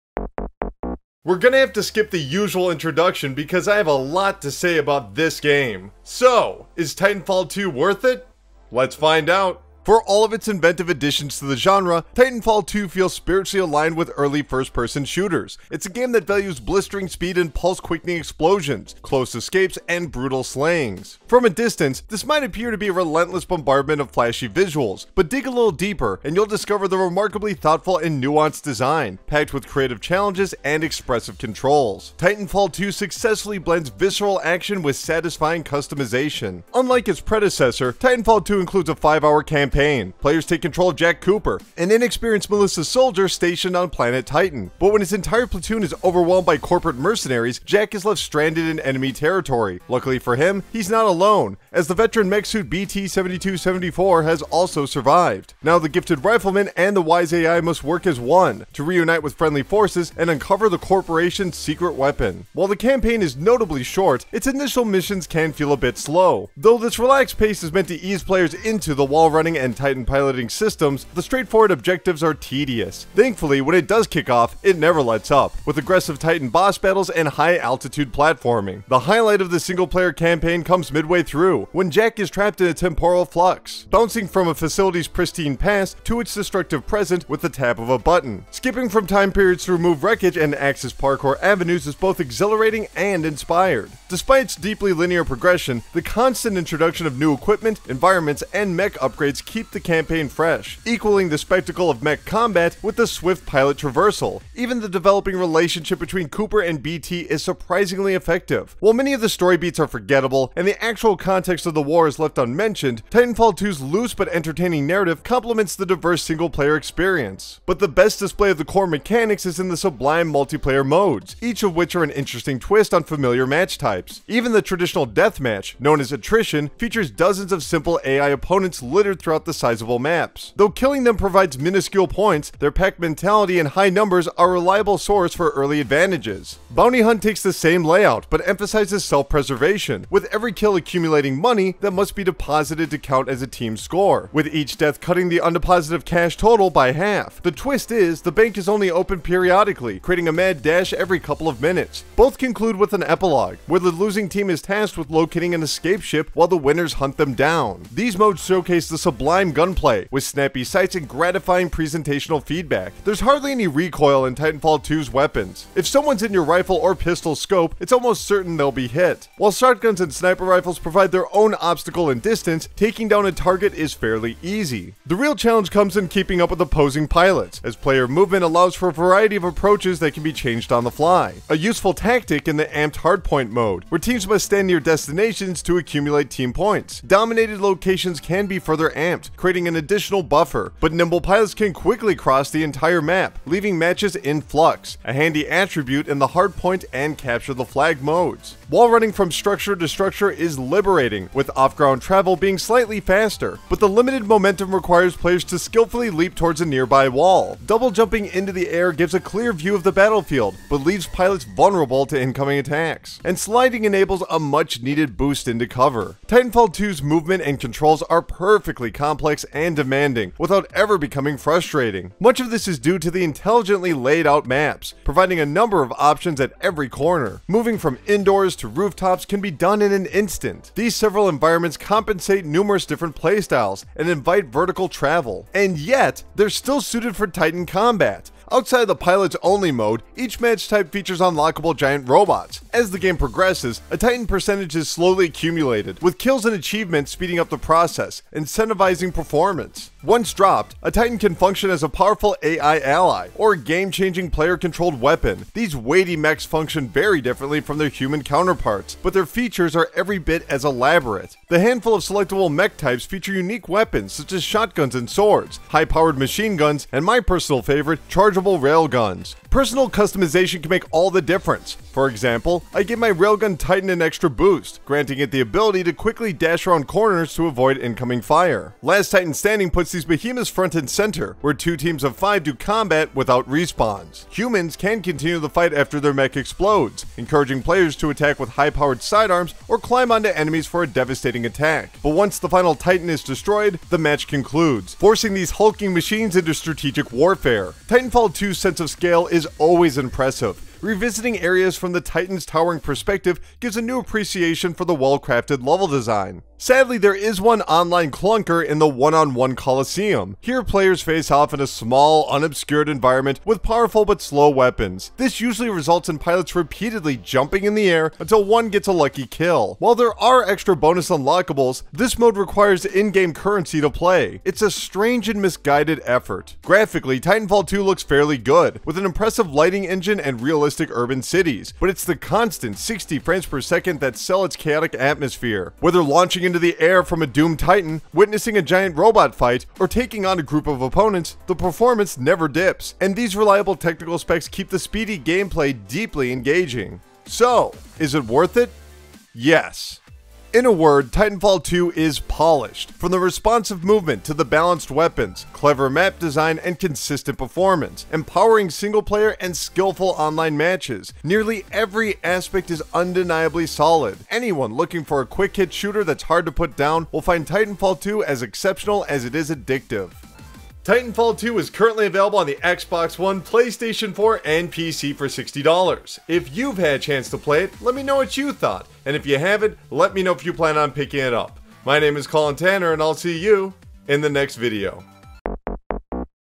Titanfall 2 [Worth It?] (PS4, Xbox One, PC)
One of the Best Games of the Year! Colin Reviews Titanfall 2 on the PlayStation 4!